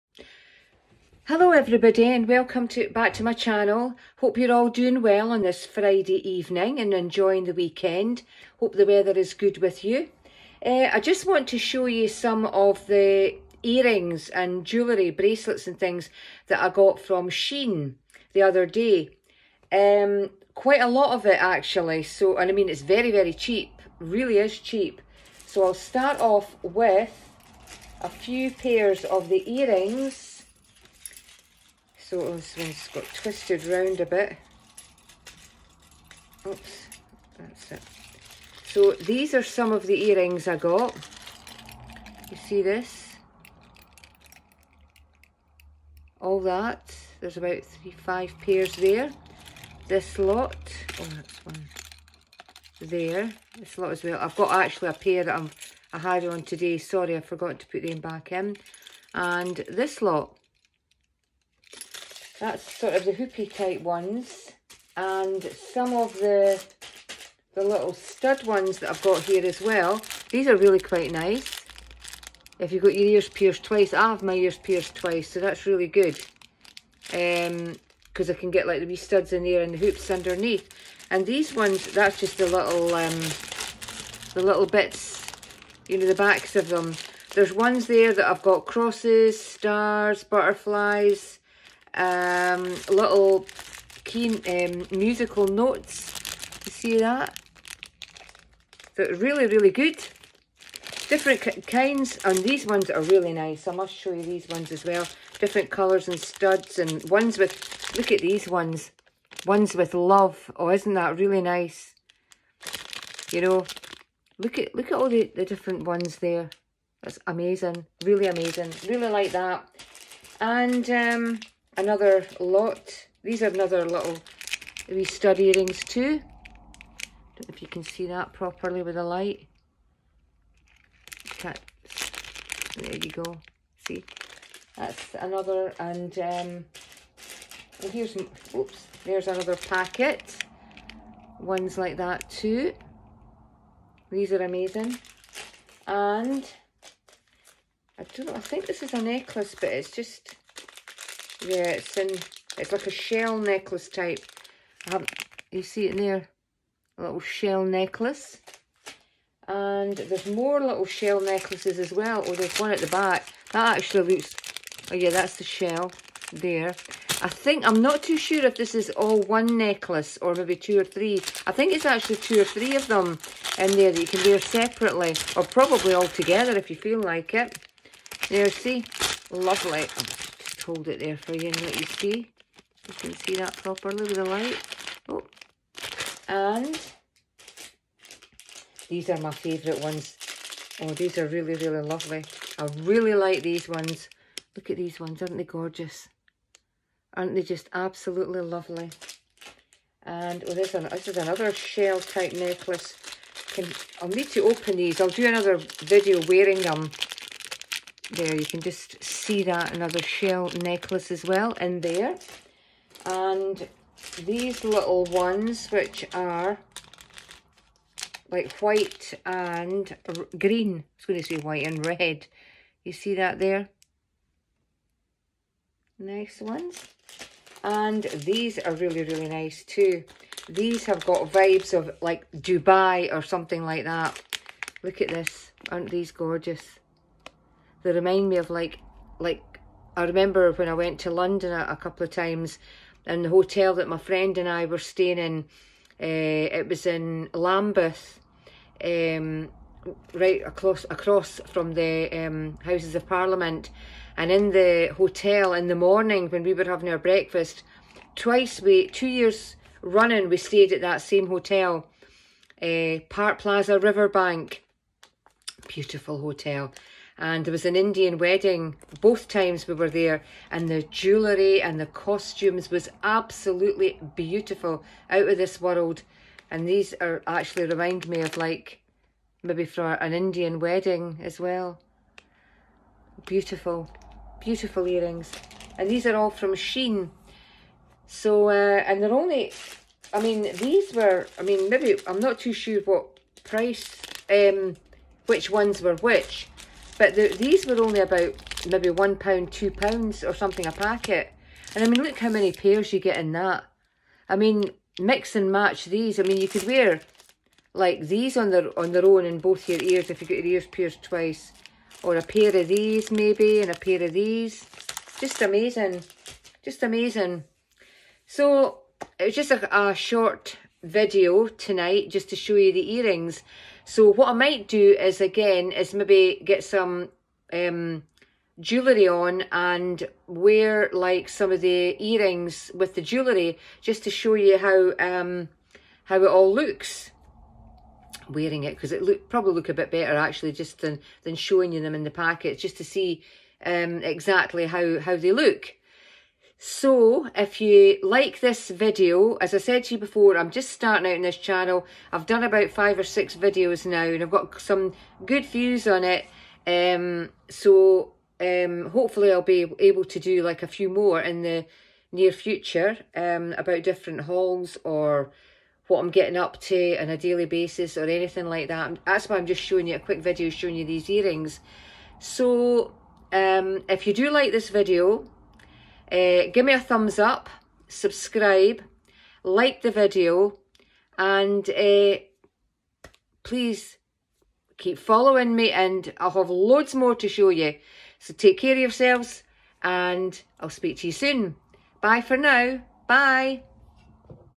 SHEIN jewellery haul chat
Chatting about my Shein jewellery plenty exciting stuff great value and lots to choose from shopping